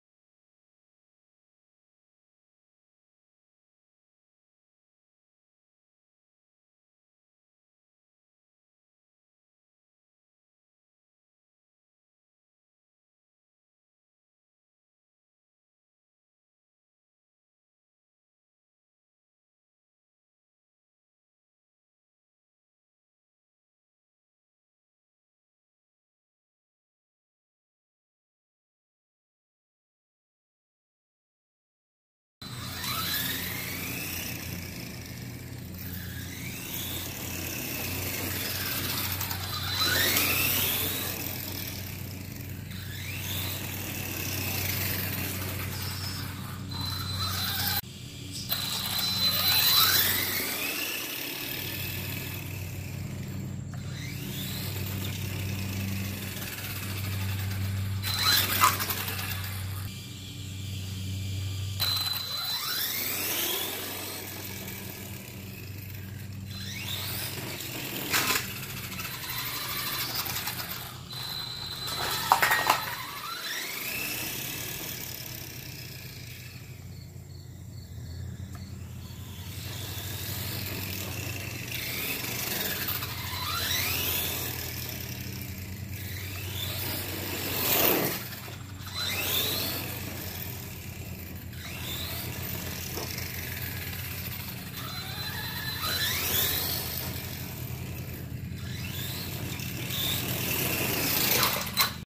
hpi mt2 6s - First Run
this little thing has been through so much. there are countless drill holes in it's chassis. first it was a .15 nitro car that had a seized engine. then it turned into a .21big block mt2, then a .28 big block mt2, and now it is running on a hobby wing 6s esc with a 50c and a 100c 3s lipo. this thing is going on the roof.